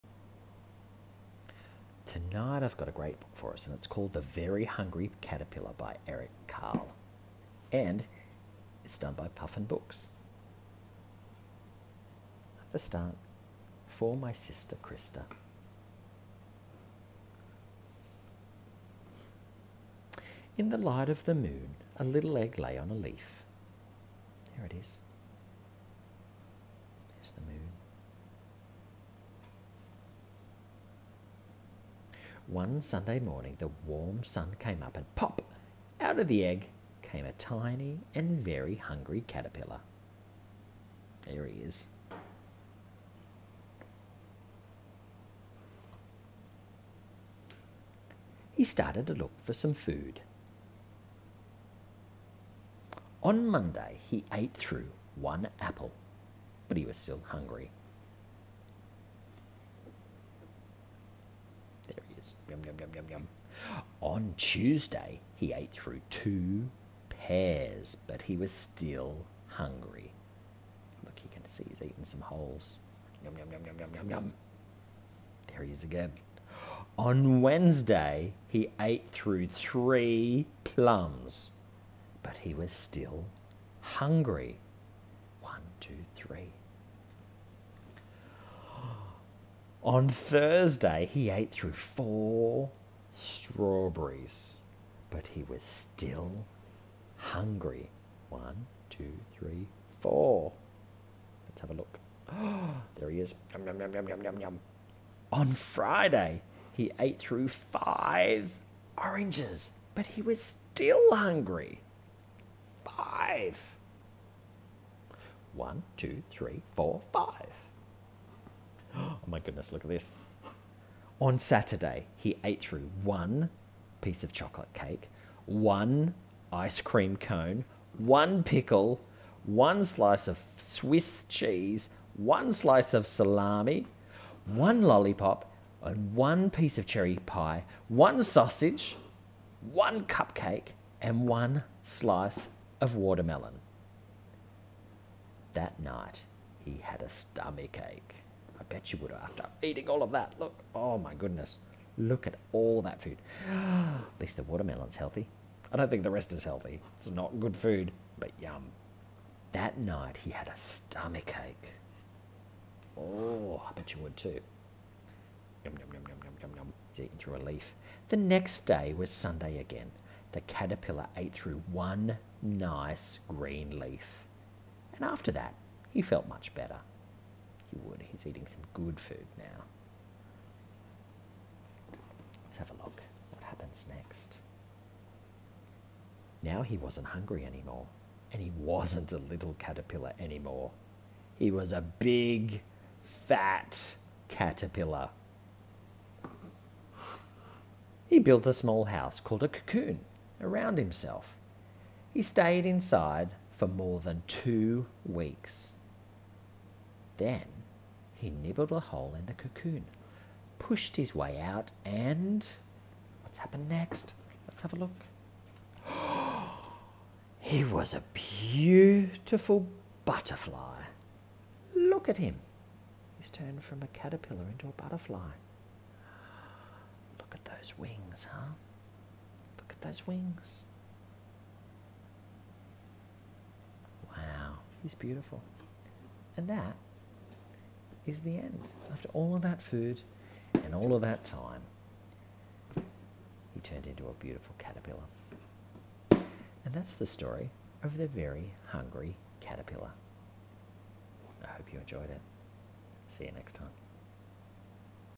The very hungry caterpillar, read aloud by a dad
A Little Miss book called, “Little Miss Wise” read aloud by a Dad.